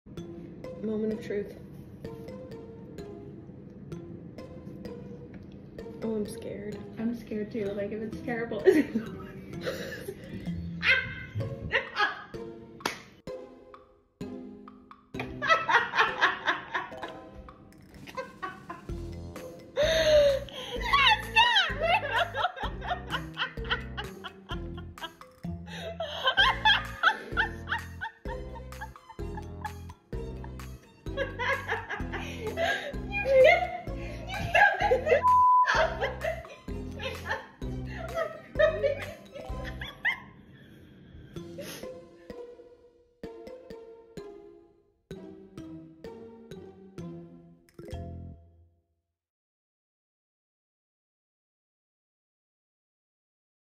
Mum opens cast of baby son's hand - to find he's giving her the finger 🤣✨ | LOVE THIS!
Credit: SWNS / Collab